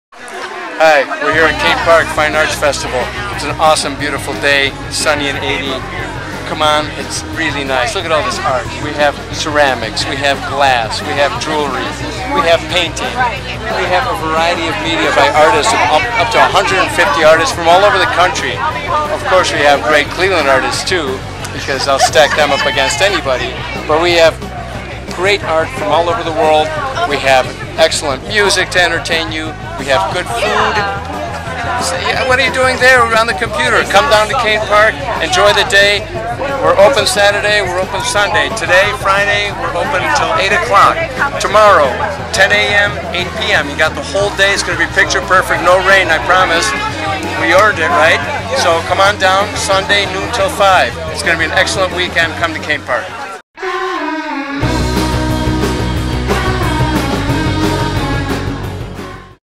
Cain Park Arts Festival 2013-Day 1
The renowned Cain Park Arts Festival begins its 35th year of showcasing fine art from across the country. July 12-14, 2013. Friday 3-8pm, Saturday 10am-8pm, and Sunday 12-5pm. Plus there's entertainment and fantastic food.